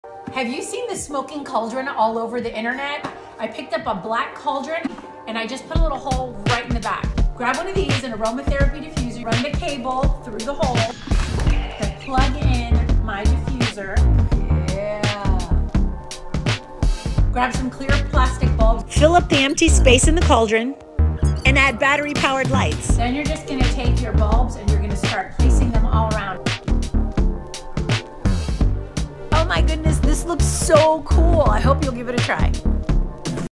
DIY Smoking Cauldron
How to create the perfect Halloween home decor piece.